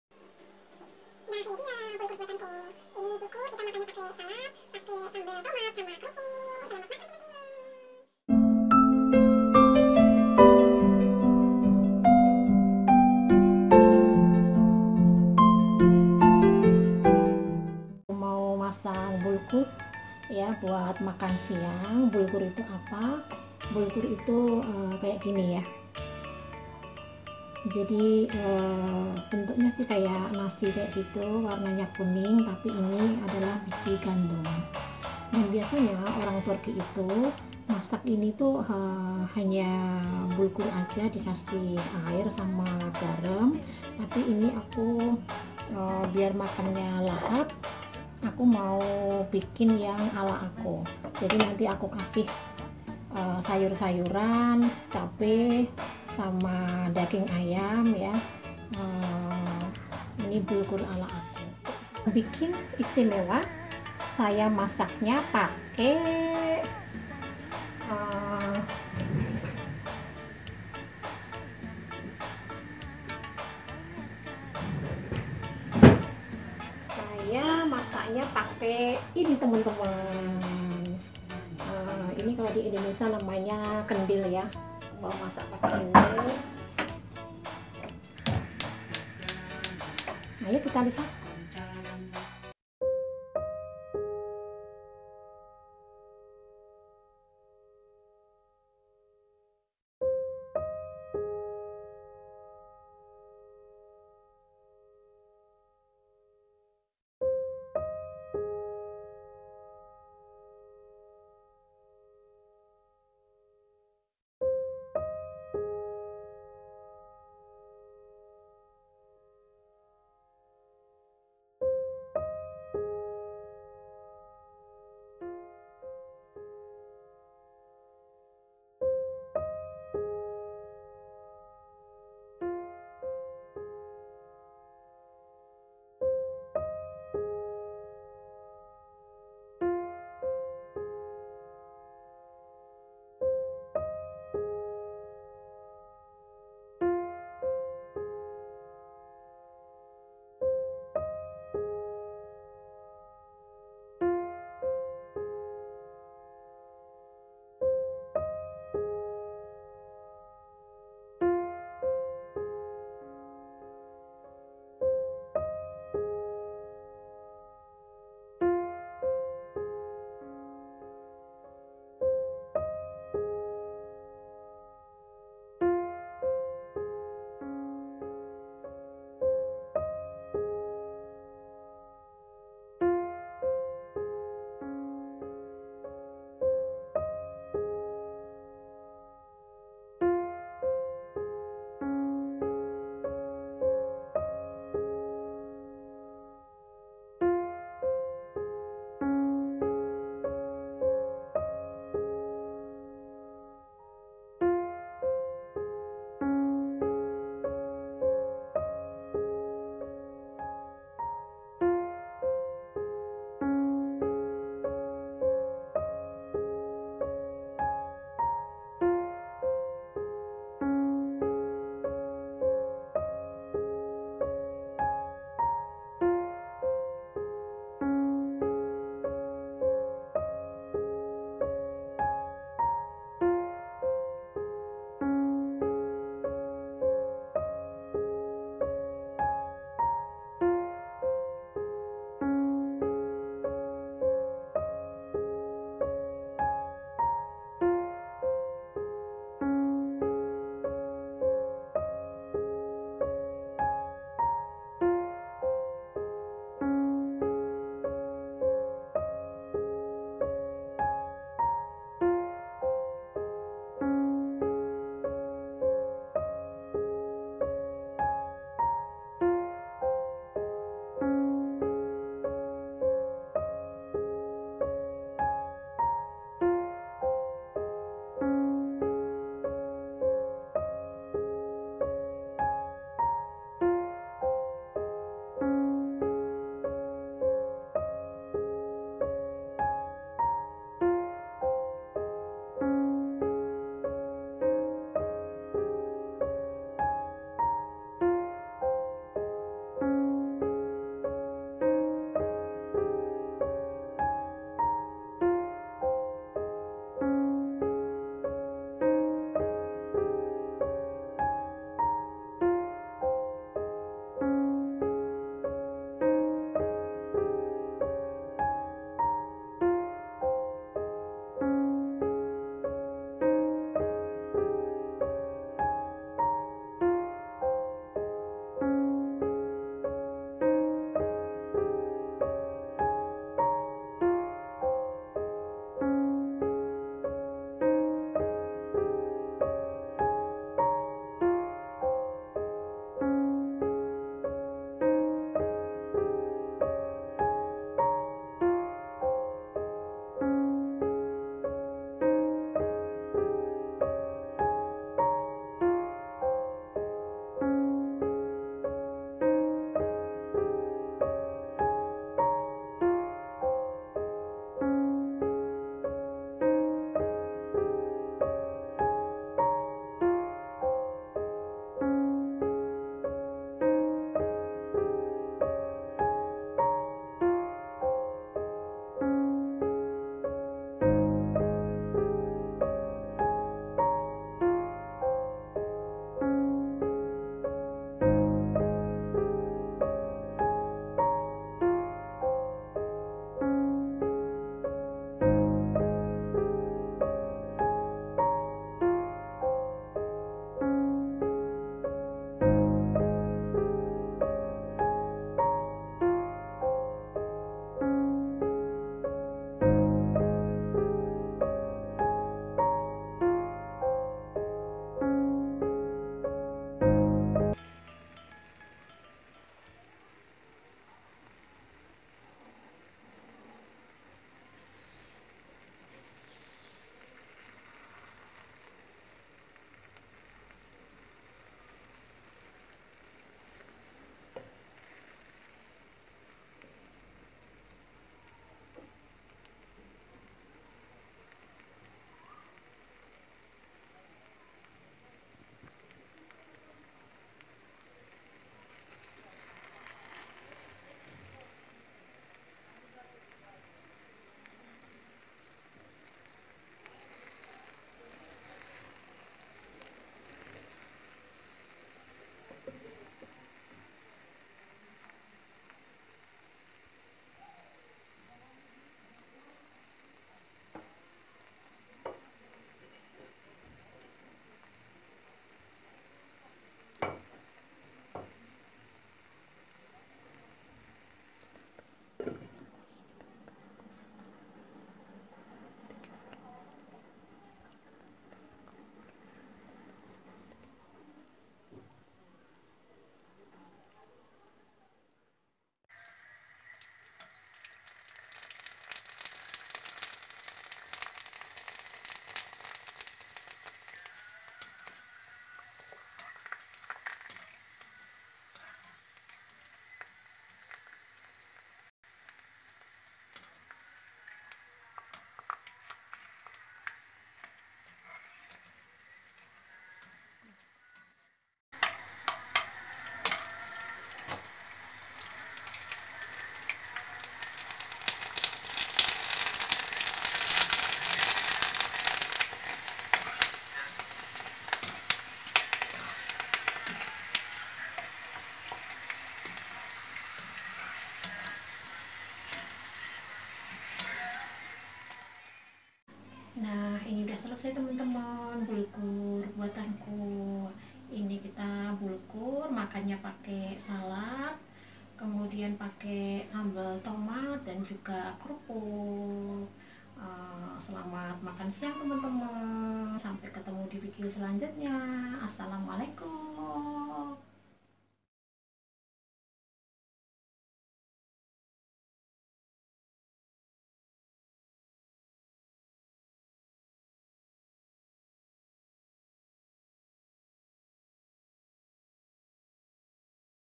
MEMBUAT BULGUR PAKAI KENDIL || TURKISH FOOD BULGUR
Assalamu'alaikum wr.wb
Hallo temen2 kali ini aku mau masak bulgur buat makan siang. Bulgur ini adalah makanan turki yang praktis, tapi saya masaknya ala aku yah biar sesuai dengan lidahku yang orang indo ini.
bahan:
1.5 gelas bulgur
3.5 air
1/4 kg daging ayam
1 buah wortel
2 buah cabe hijau
1 buah tomat
bumbu:
1 siung bawang putih
1 bawang bombay
1 sdt mrica
1 sdt bubuk cabe
1 sdt daun mint
1.5 sdt garam
cara membuatnya:
tumis bawang bombay dan bawang putih sampai harum, kemudian masukan cabe hijau dan ayam tumis sampai ayam agak kaku, kemudian masukan wortel, tomat, bulgur dan air. masak sampai mendidih.
setelah mendidih masukan bubuk cabe, mrica, daun mint dan garam. Masak sampai air meresap. Setelah air meresap matikan aduk2 sebentar lalumatikan kompor. tutup lagi dan biarkan 15 atau 20 menit.
selamat mencoba!!!

Asma levo